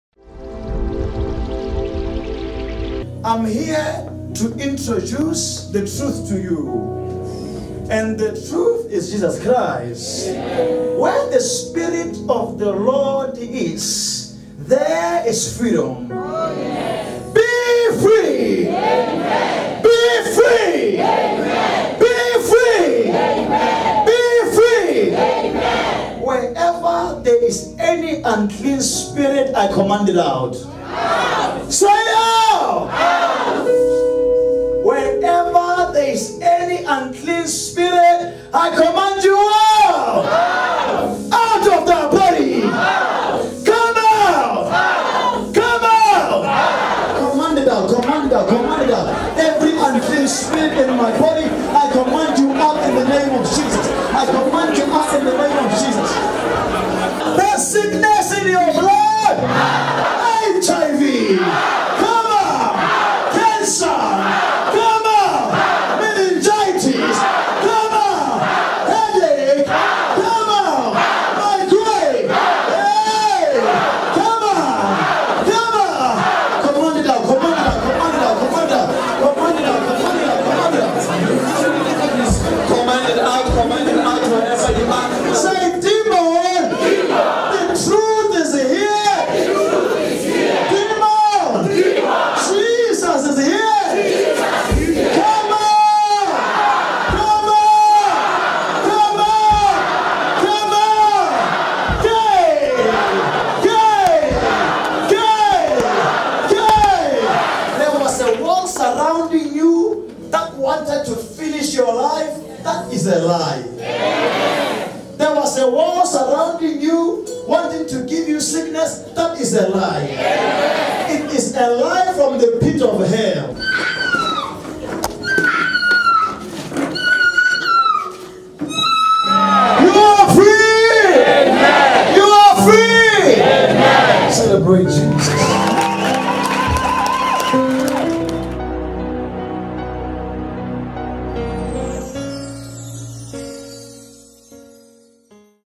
PRAY ALONG WITH PROPHET KARABO &RECEIVE THE SPIRIT OF TRUTH!
As you pray along with Prophet prepare your heart to receive the spirit of Truth. Know that your testimony is sure.
You are Chosen by God's Grace